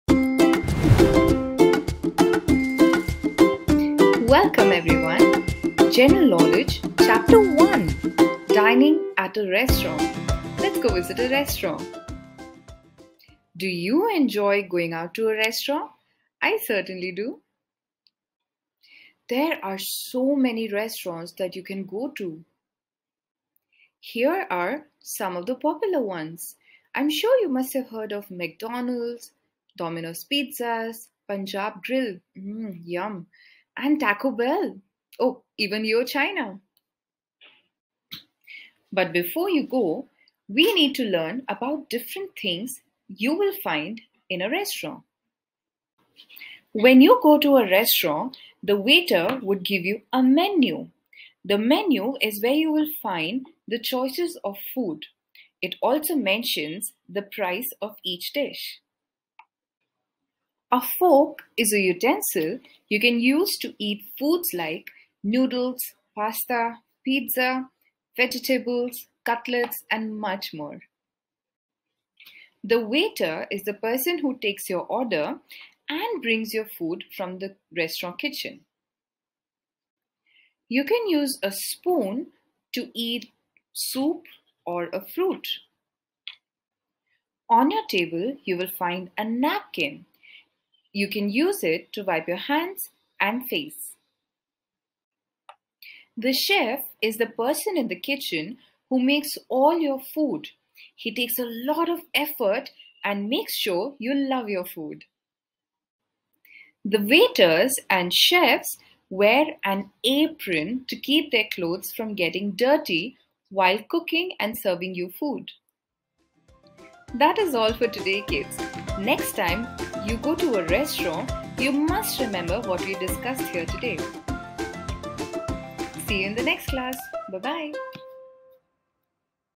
Dining at a Restaurant | General Knowledge | Class 2 | Chapter 01
Do you enjoy going out to a restaurant to eat delicious food? Well, I am sure you all do. So here is a video to teach you more about what you will get to see in a restaurant.